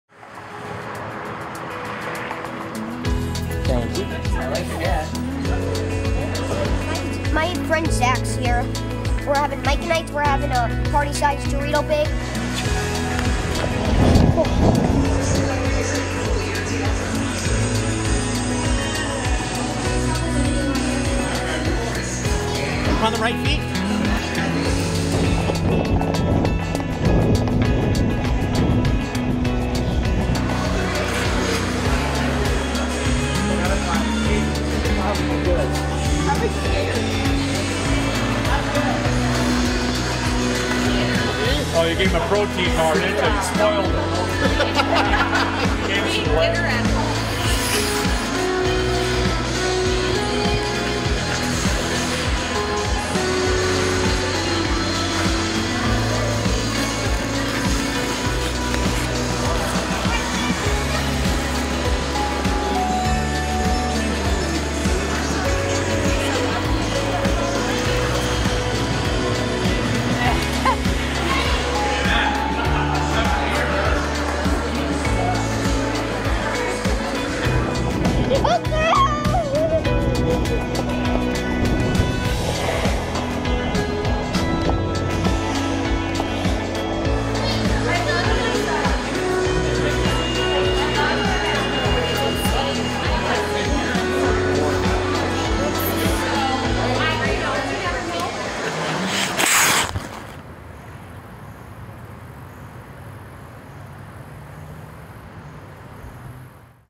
White Bear Lake Sports Center
White Bear Lake city officials enjoy an exclusive skate inside the newly remodeled, White Bear Lake Sports Center.